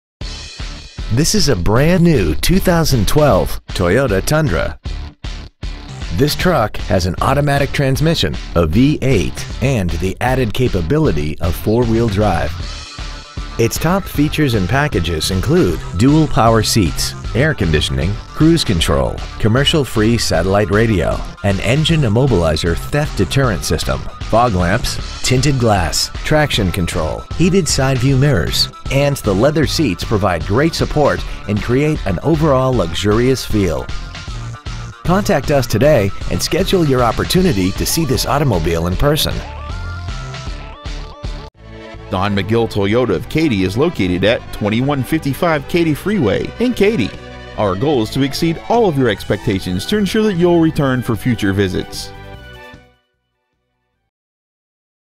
2012 Toyota Tundra 4WD Katy TX
Year : 2012
 Make : Toyota
 Model : Tundra 4WD
 Engine : 8 Cyl.
 Trans . : Automatic
 Exterior : Magnetic Gray Metallic

 Interior : Red Rock/Black
 Stock : K32193

 2012 Toyota Tundra 4WD Katy TX